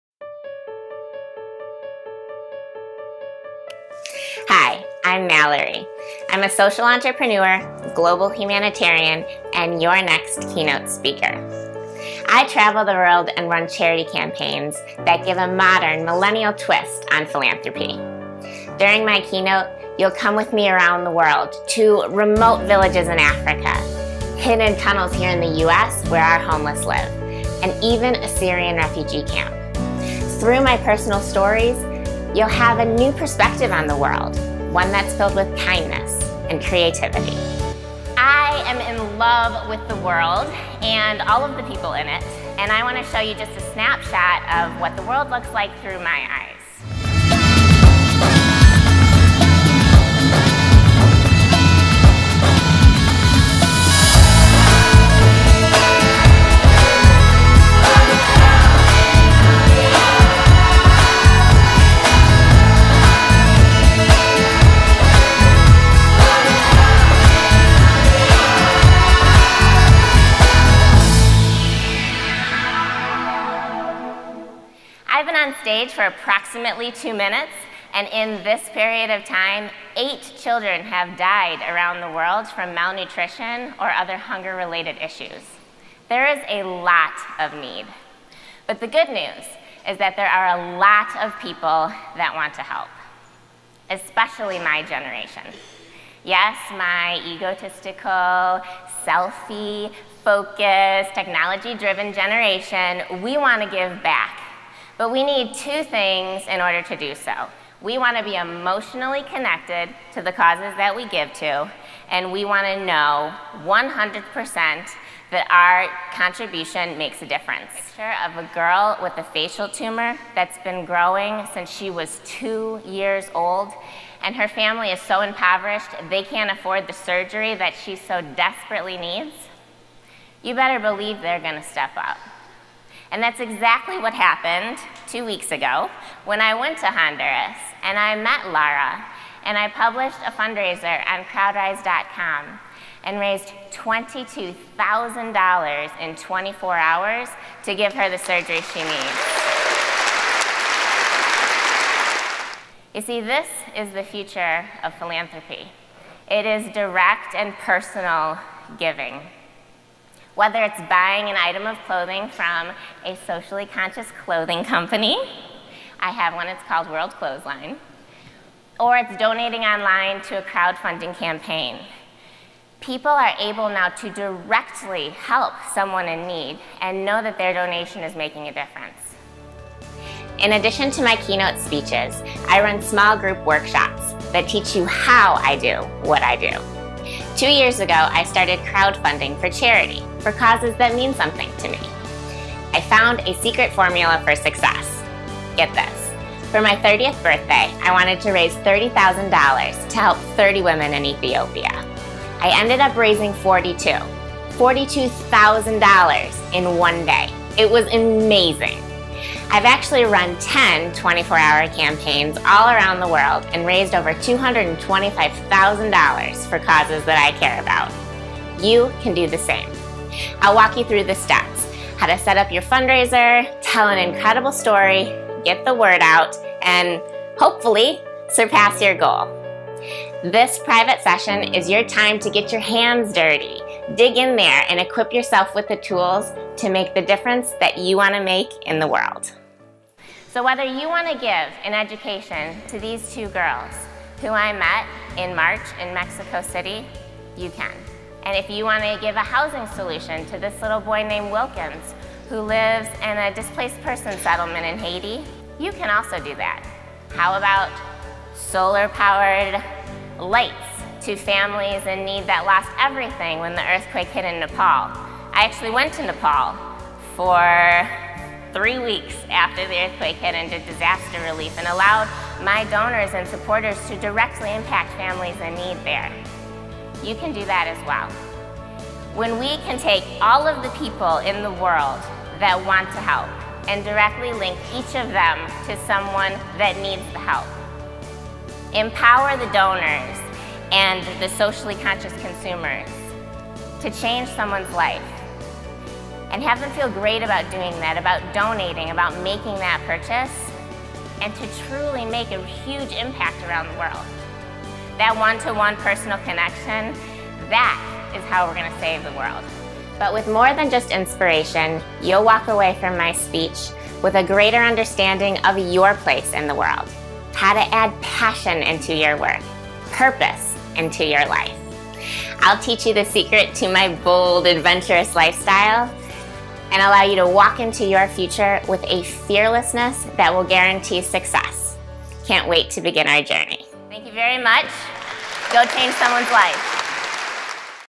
Mallory Brown Keynote Speaker, Humanitarian, Social Entrepreneur, World Adventurer
Curious about the world around her, Mallory Brown, at the age of 20, strapped on a backpack and set out to discover new people and places.  She fell in love with the world.  She learned to appreciate different ways of life and was deeply touched the universality of basic human need.

Ten years later, Mallory has become a successful social entrepreneur, worldwide adventure traveler, and humanitarian.  She is founder of World Clothes Line – a “buy 1, give 1” apparel company on a mission to clothe the world.  Mallory is the first brand ambassador for CrowdRise, the largest crowdfunding platform for good.  As director of the CrowdRise 24-Hour Impact Project, she runs flash fundraisers that engage donors in full-circle philanthropy.  Mallory also serves as Director of Communication for global non-profit Chefs4Kids.  She creates innovative strategies for giving and transforms the lives of children in need around the world.

Mallory has worked on-the-ground in 22 countries, representing dozens of companies, and hundreds of generous donors to help thousands in need.  Her efforts have been featured by The Today Show, the New York Times, Crain’s Detroit Business, the Huffington Post, and Ambassador Magazine.  Mallory has attracted corporate sponsorship from organizations such as Ford Motor Company, Zappos, Moosejaw Mountaineering, CreateMyTee, and Meijer.  Mallory shared her new approach to philanthropy at TedxDetroit 2015 with a talk titled “How to Save the World Before You’re 30.”

Mallory was born and raised in Michigan and currently lives on the edge of Detroit.  She believes that direct, personal giving will inspire the next generation to give back.  Mallory’s unique approach to charity is making a mark in the philanthropic world and proves that each person truly can make a difference.